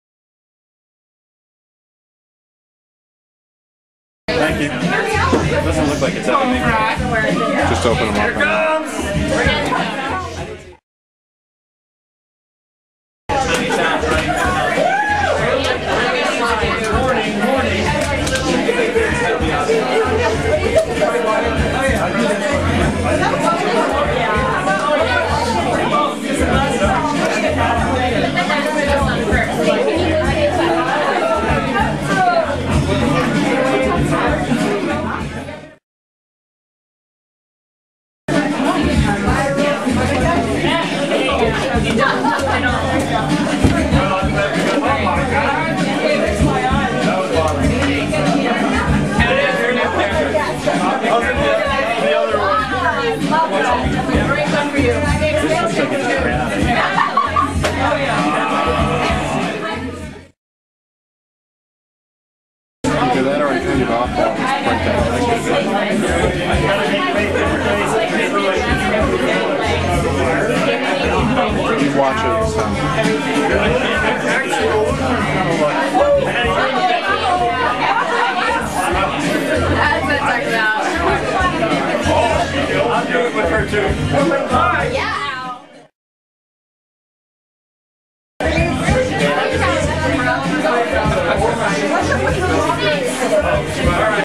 Chloe and Karin at the Photo Booth
This video is of Chloe and Karin using the photo booth at Scott's Wedding.